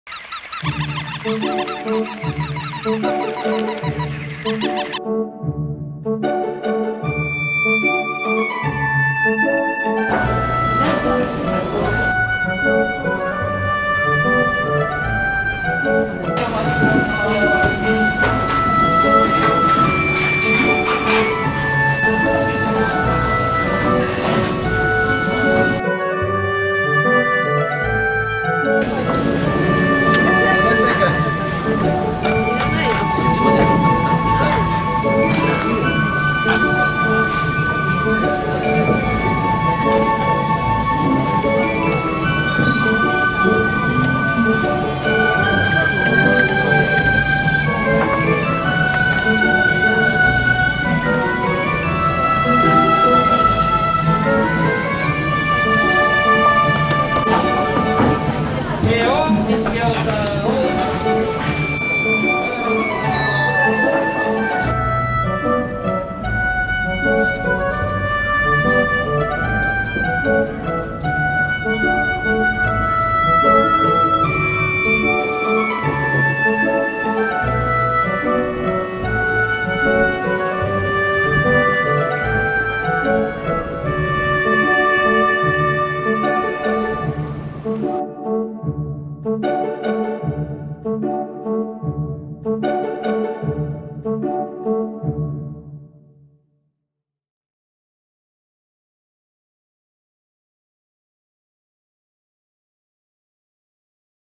【漁港直営食事処ばんや】【南房総】人気の食事処ばんやのおススメ大漁御膳はネタがたっぷり。アナゴなど朝獲れの魚介類で旨い。
南房総鋸南町食事処ばんやは大人気。お値段納得の漁師料理を頂けます。大漁御膳の紹介です。

コメントも歓迎します。